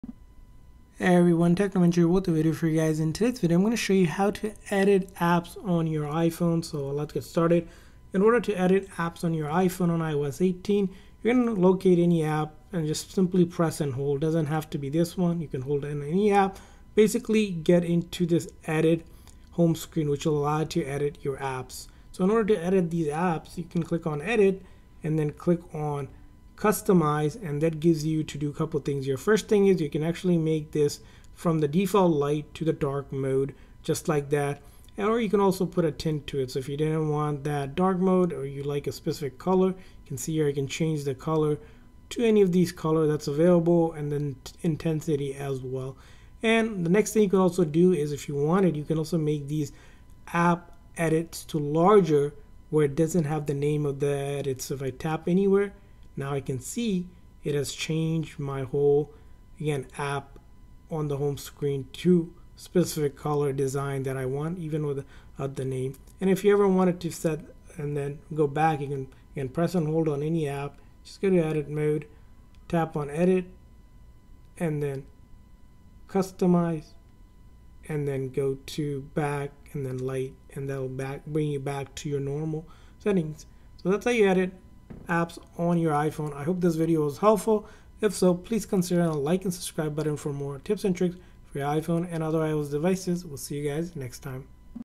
iOS 18 How To Edit Apps
In this tutorial how to edit and customize apps on iPhone on iOS 18!